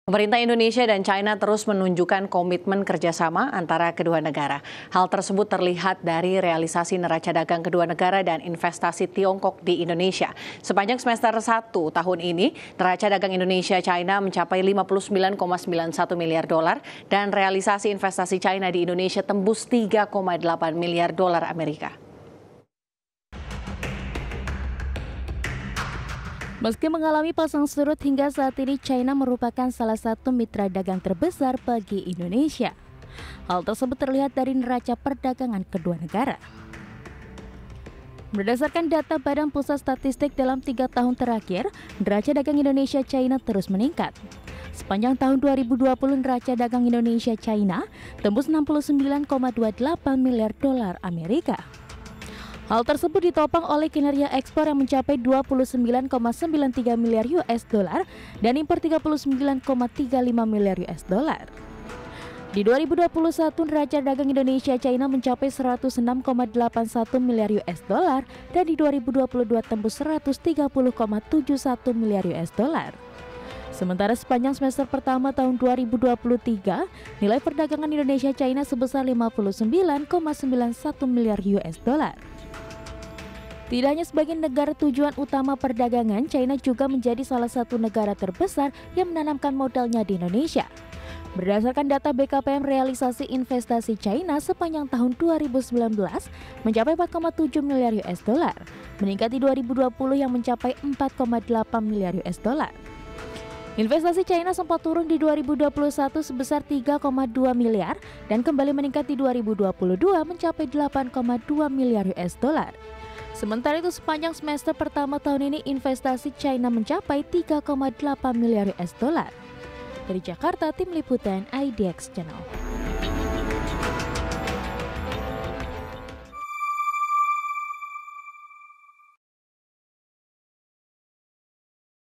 Penguatan Kerja Sama Indonesia-China | IDX CHANNEL
Pemerintah Indonesia dan China terus menunjukkan komitmen kerja sama antara kedua negara. Hal tersebut terlihat dari realisasi neraca dagang kedua negara dan investasi China di Indonesia sebanyak Semester I-2023, neraca dagang Indonesia China mencapai 59,91 miliar dolar dan realisasi investasi China di Indonesia tembus 3,8 miliar dolar Amerika.